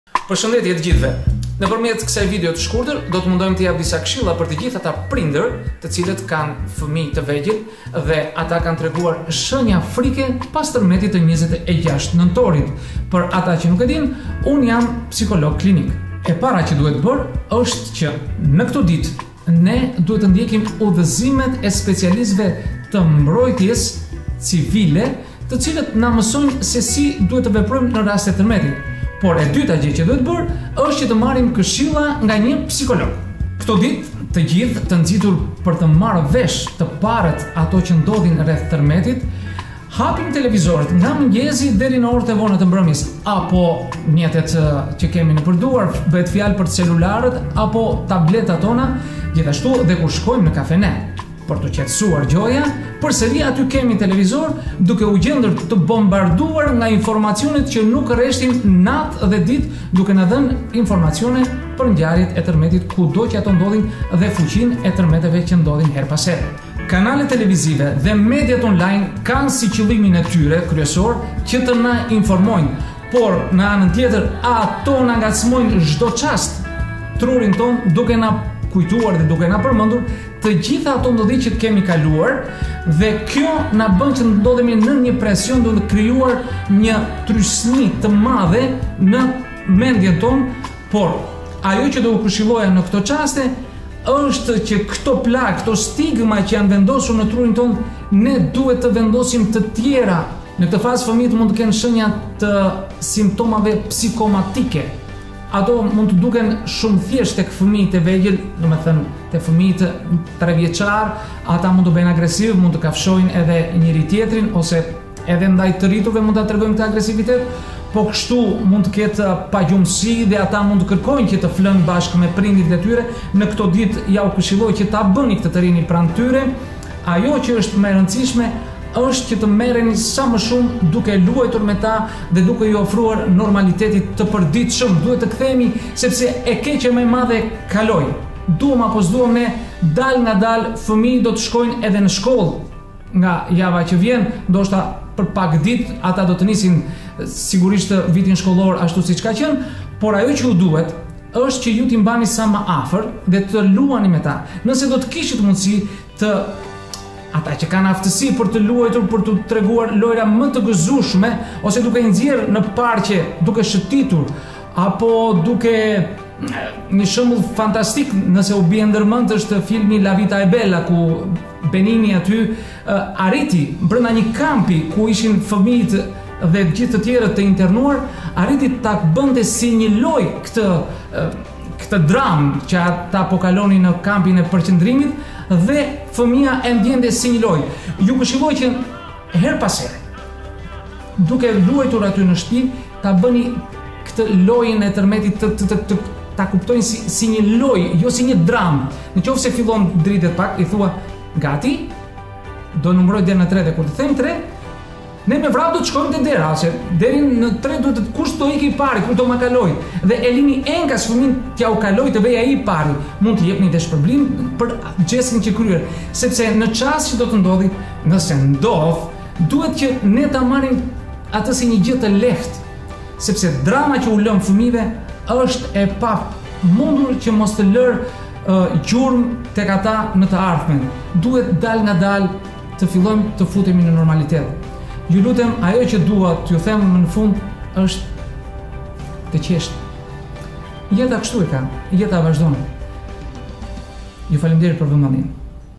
Këshilla psikologjike për prindërit për fëmijët pas tërmetit të 26 nëntorit
Përshëndetje! Në këtë video të shkurtër po jap disa këshilla se si prindërit mund të ndihmojnë fëmijët e tyre te cilët jane tronditur nga tërmeti i 26 nëntorit 2019.

Për me shumë dije shikoni videon

 Ju falënderoj!